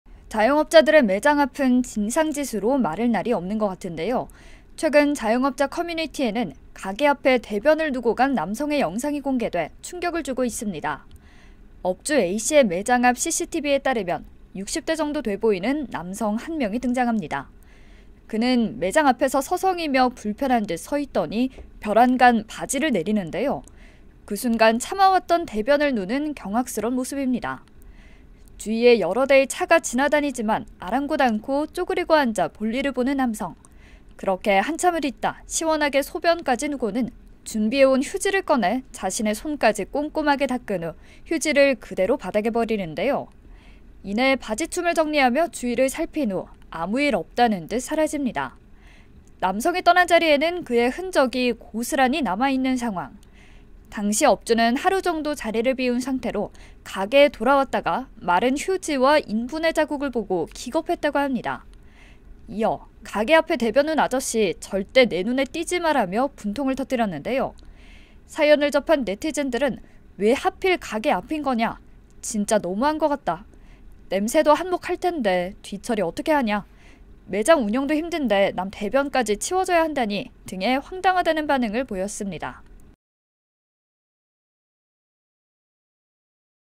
남의 가게 앞에 쪼그리고 앉더니... / KNN
자영업자들의 매장 앞은 진상 짓으로 마를 날이 없는 것 같은데요.